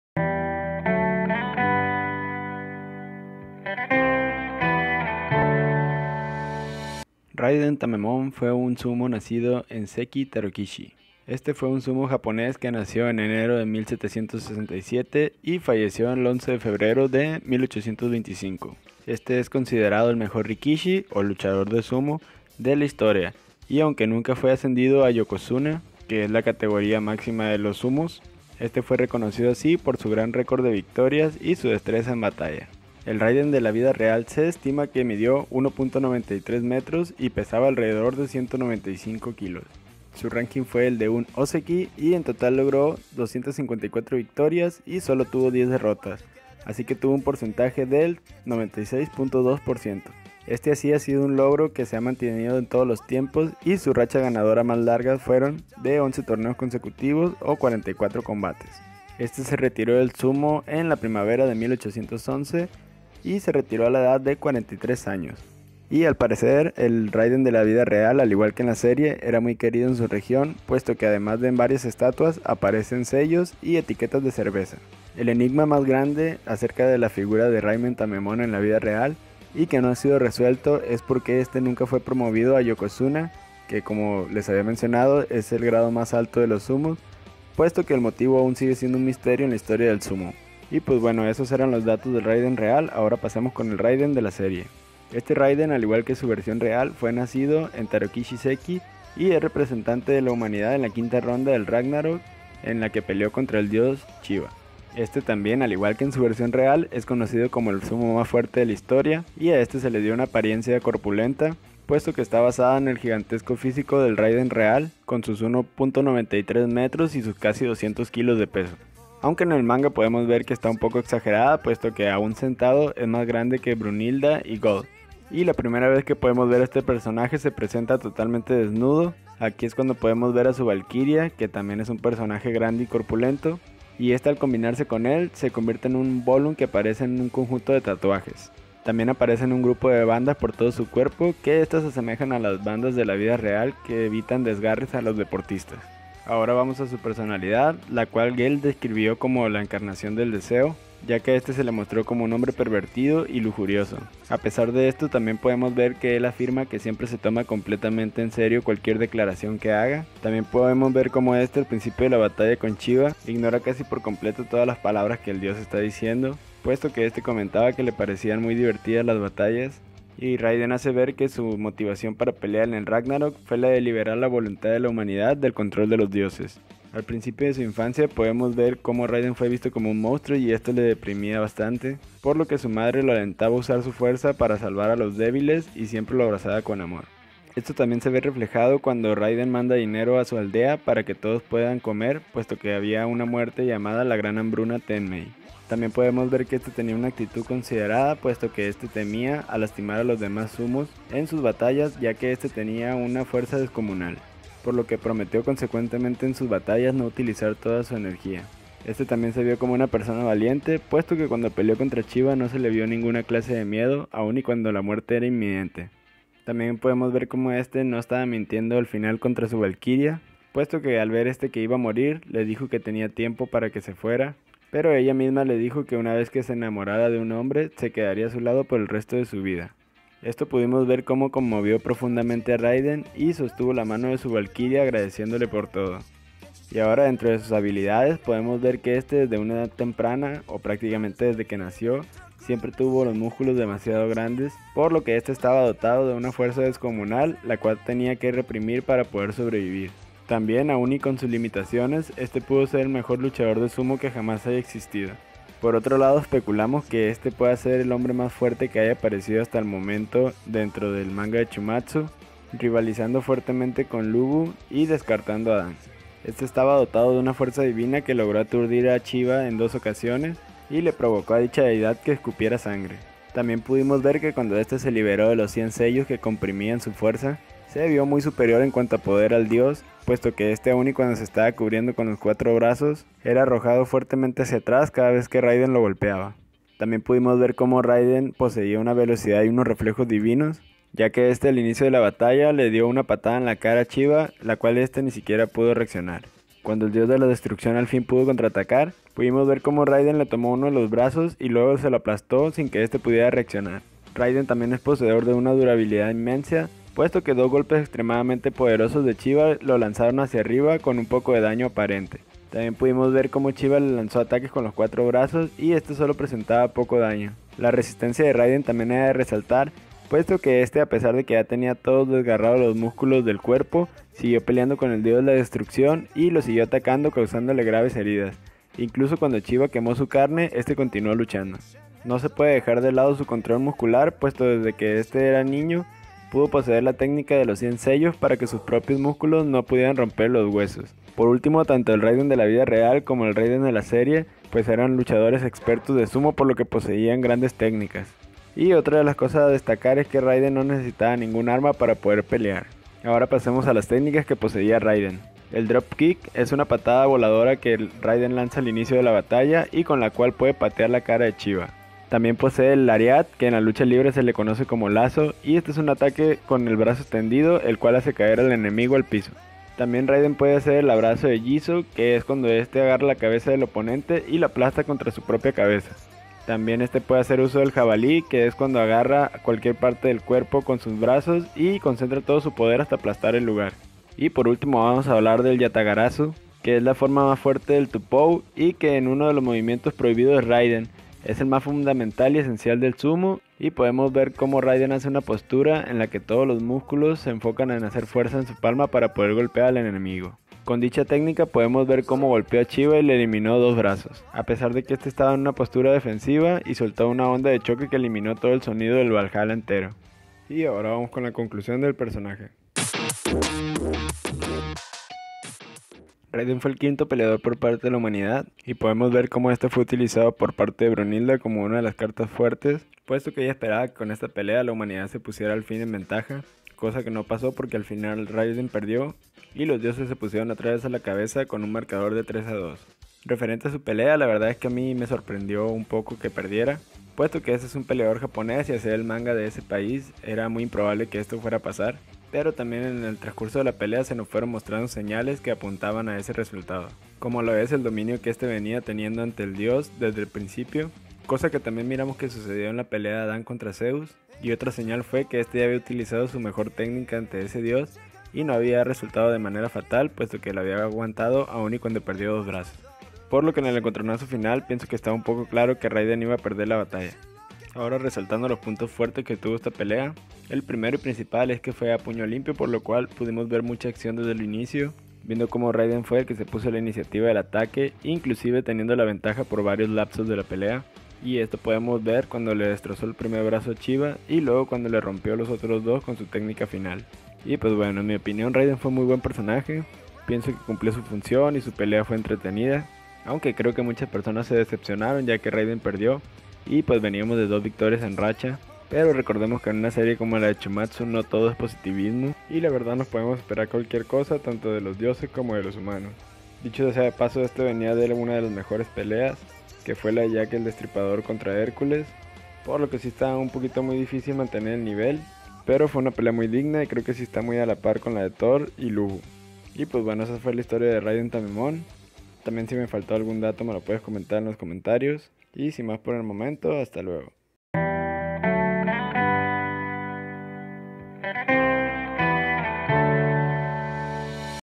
Raiden Tameemon Shuumatsu No Valkirye | Record Of Ragnarok
Historia, Reseña y Resumen del Raiden Tameemon real y de la serie